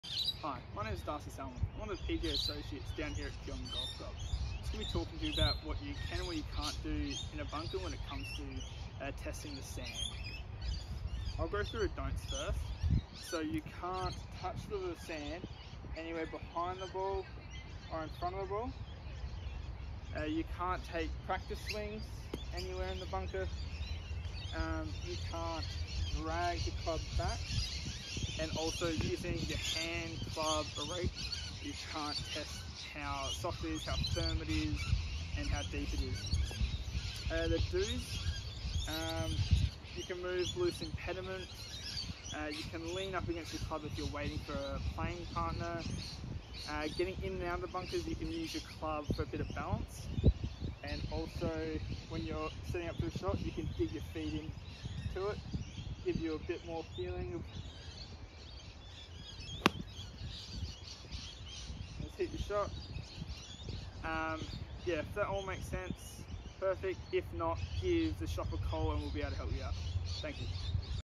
Do's and Don'ts in the Bunker
Darcy Salamon, PGA Trainee at Kooyonga Golf Club demonstrates the do's and don'ts while in a bunker